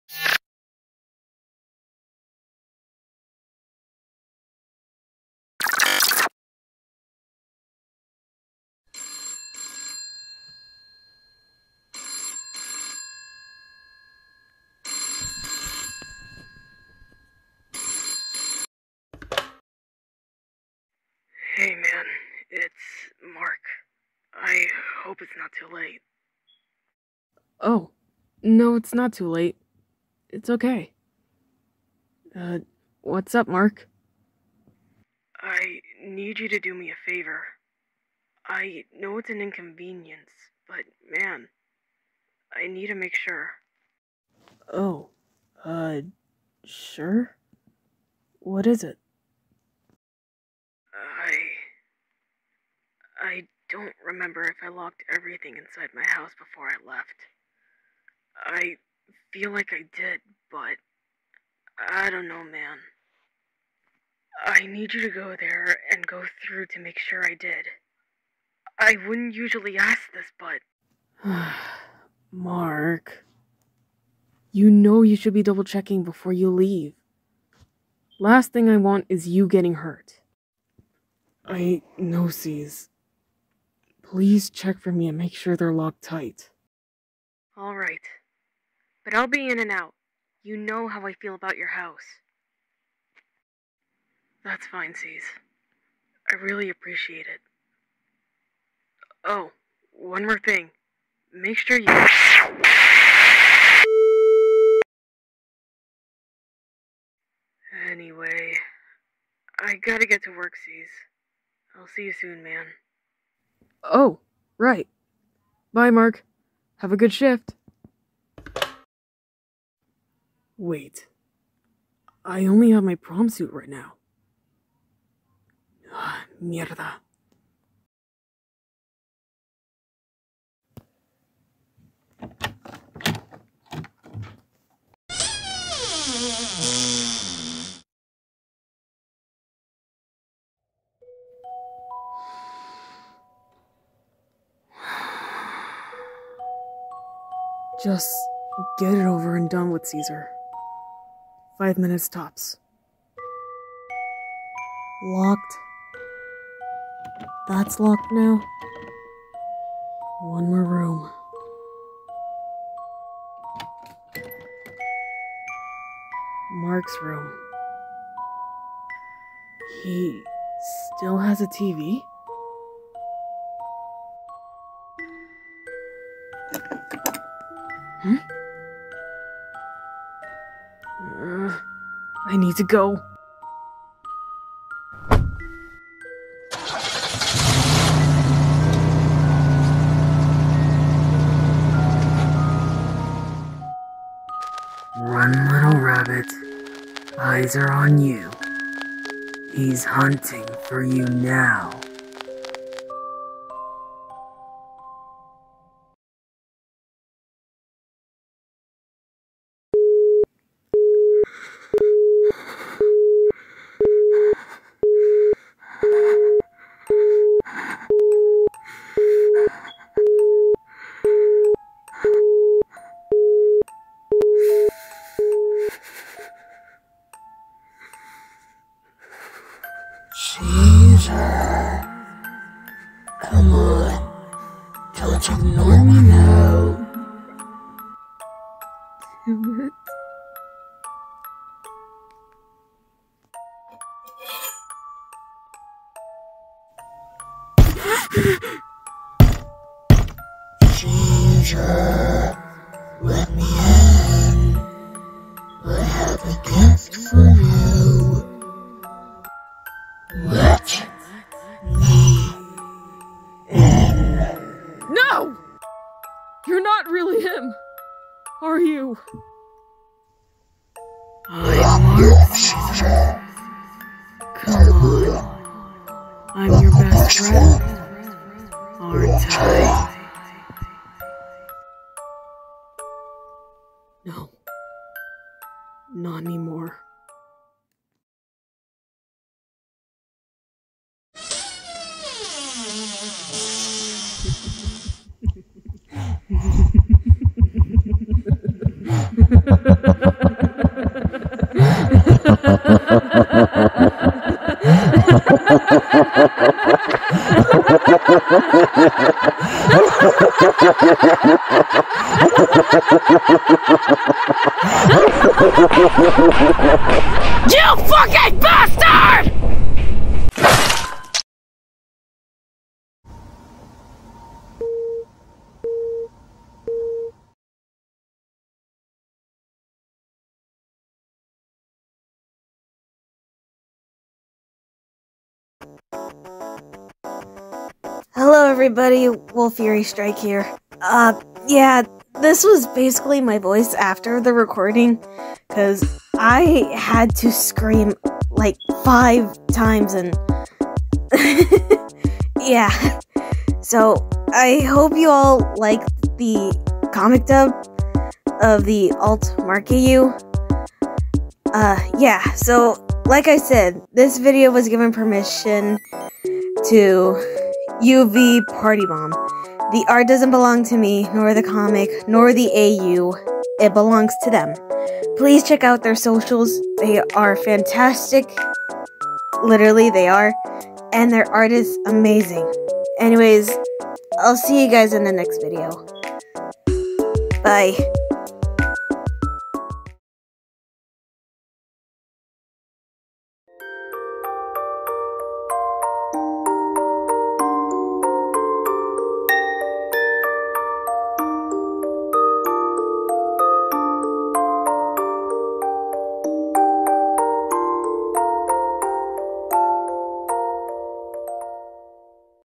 Alternate Mark Heathcliff //Comic Dub// The Mandela Catalogue (A Mandela Catalogue AU) Part 2
A new Alt Mark Comic Dub!! I may have broken my voice while doing this but at least it was worth it, I'm starting to get the hang of CapCut and it's awesome, and actually more easy than the other editing apps I used!

This video was given permission by the original creator of the Alt Mark AU, UVPartyBomb! Please go check them out, they make fantastic art and there comics are amazing!

Programs I used:
CapCut
Audio Recorder
Video to mp3 downloader

Some of the other sound effects were made by me

The original creator of The Mandela Catalogue: @AlexxKister

Programs I used for the outro:
KineMaster
Flipaclip

People in the video:
Human?Alternate¿ With a grey hoodie: Mark?¿ Heathcliff
Boy with suit and tie: Cesar Torres

People in the outro:
Light blue night fury wolf: Wolf Fury
Adorable green and yellow two tailed fox: My bf David